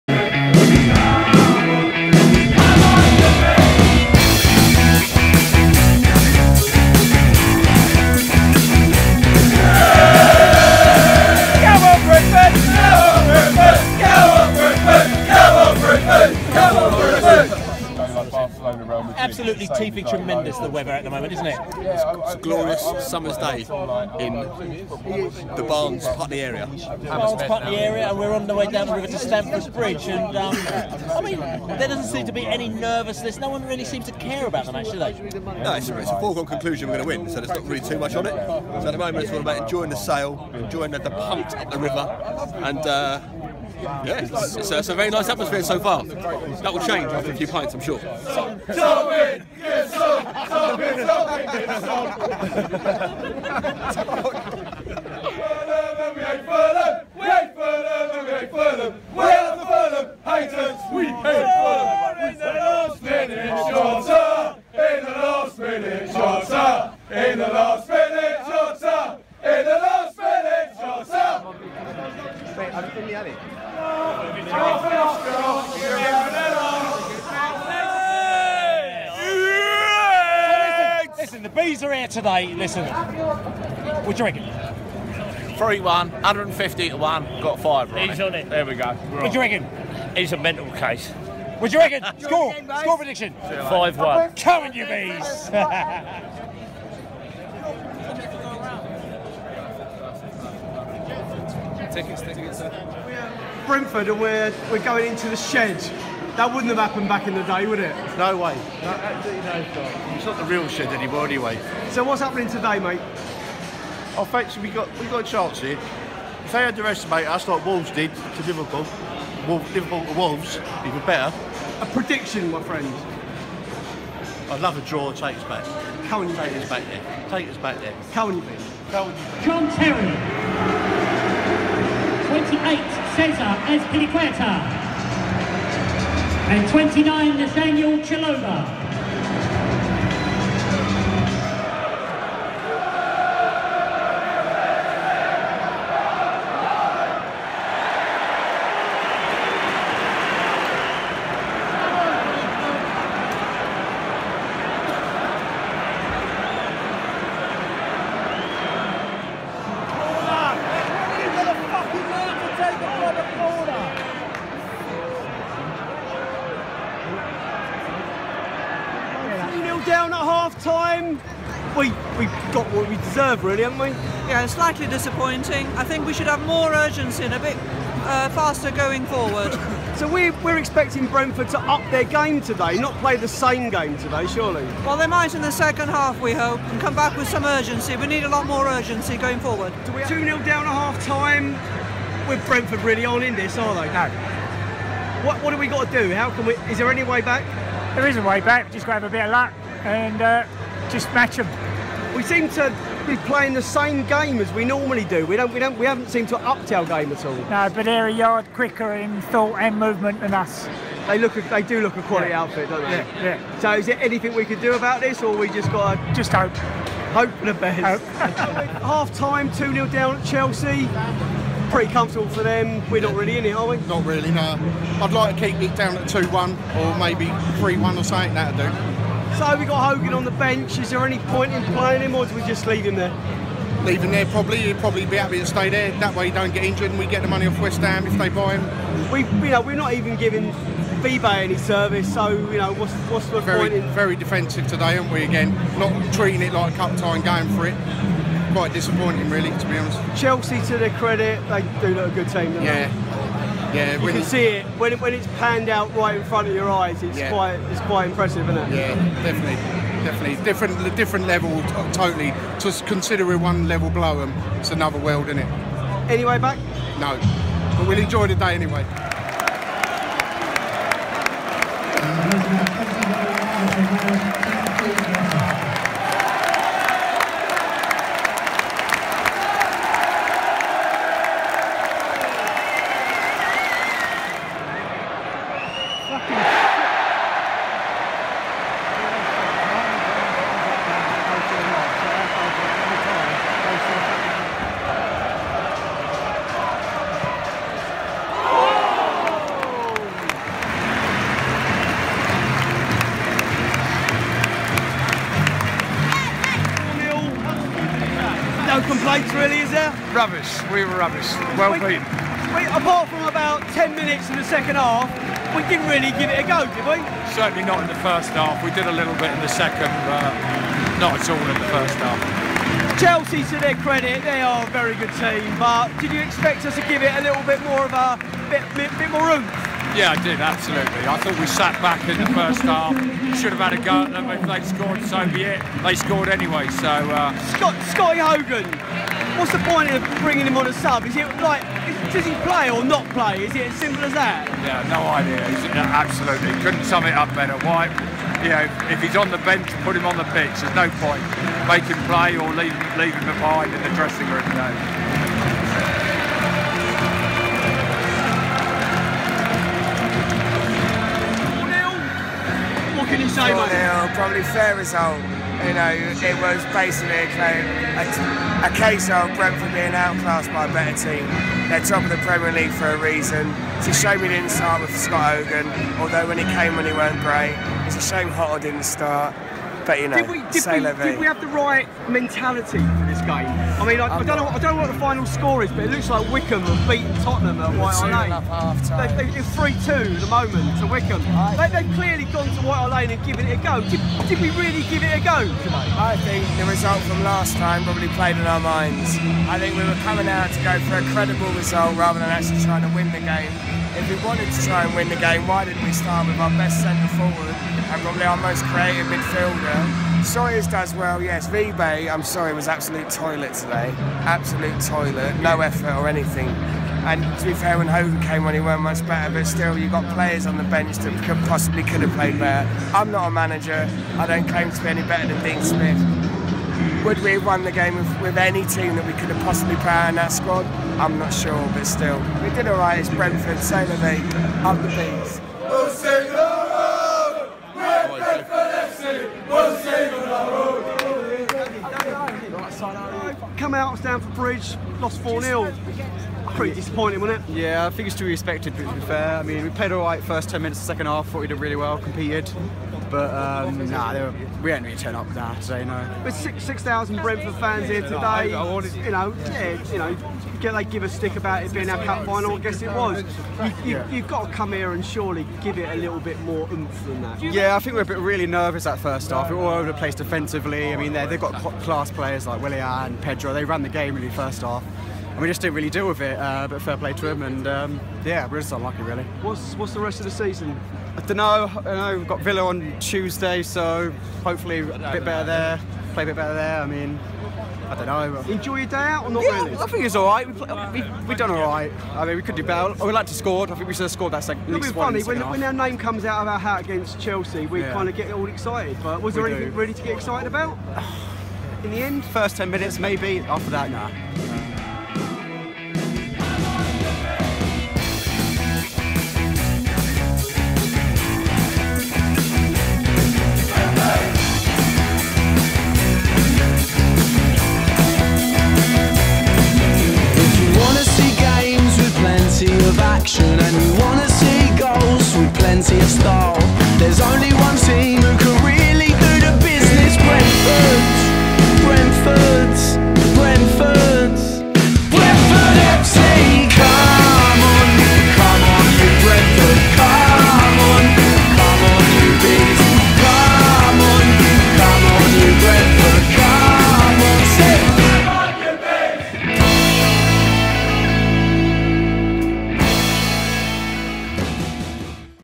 Chelsea 4 Brentford 0
Brentford went to Stamford Bridge hoping to pull off an FA Cup shock. Here's all the pre, during and post-match thoughts and opinion from Bees and Blues fans.

Filmed and Edited by Billy Grant and Dave Lane